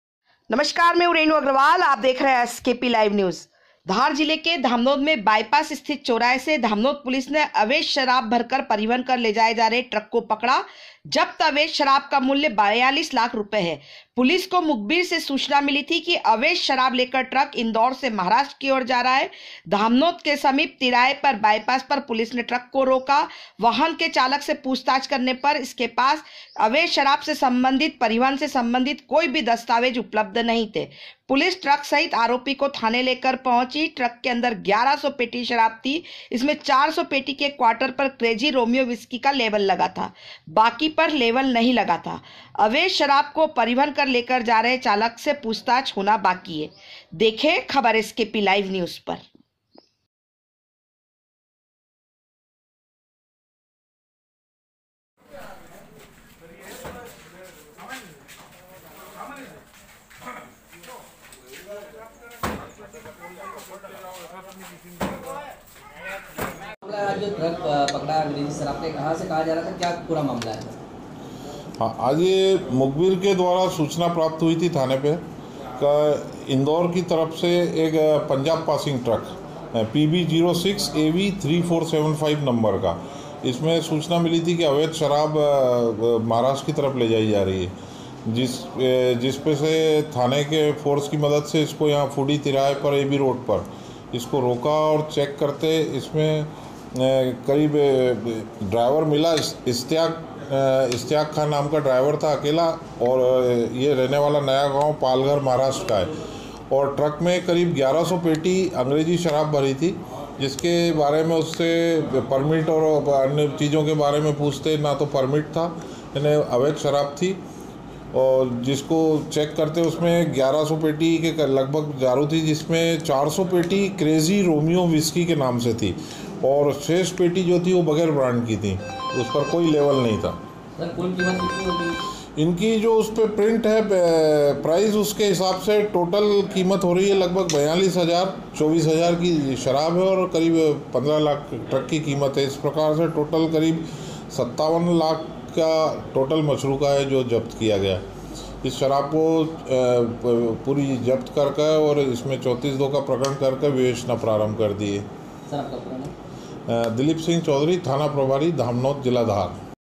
धामनोद। 42 लाख की शराब ,अवैध परिवहन कर महाराष्ट्र की और ले जाई जा रहे ट्रक को police ने पकड़ा
रेणु अग्रवाल
धार जिले के धामनोद  में फूडी होटल बायपास स्थित चौराहे से धामनोद पुलिस ने अवैध शराब भरकर ले जाए जा रहे ट्रक को पकड़ा जप्त  अवैध शराब 42 लाख रुपए मूल्य की है ।
पुलिस को मुखबिर से सूचना मिली थी कि अवैध शराब लेकर ट्रक इंदौर से महाराष्ट्र की ओर जा रहा है धामनोद के समीप फूडी तिराहे पर  बाईपास पर पुलिस ने ट्रक को रोका वाहन के चालक से पूछताछ करने पर उसके पास इस अवैध शराब के परिवहन संबंधित कोई भी दस्तावेज उपलब्ध नहीं थे ।
पुलिस ट्रक सहित आरोपी को थाने लेकर पहुंची ।
तिरपाल को खोला गया ट्रक में शराब की पेटियां भरी हुई थी आपको बता दें कि शराब की गंध ना आए इसके लिए ट्रक में शराब की पेटीयो के बीच फिनाइल के पाउडर को रखा गया था पकड़ी गई शराब के वाहन को खाली कराकर थाना परिसर में बने गोदाम में रखवाया गया है ।
ट्रक के अंदर 1100 पेटी शराब थी ।
इसमें 400 पेटी के क्वार्टर पर क्रेज़ी रोमियो व्हिस्की का लेबल लगा था बाकी पर लेवल नहीं लगा था अवैध शराब को परिवहन कर लेकर जा रहे चालक से पूछताछ होना बाकी है जप्त की गई शराब का प्रिंट रेट के अनुसार मूल्य 42 लाख 24 हजार है वहीं जपत ट्रक की कीमत 15 लाख है ।
बाइट ti धामनोद दिलिप चौधरी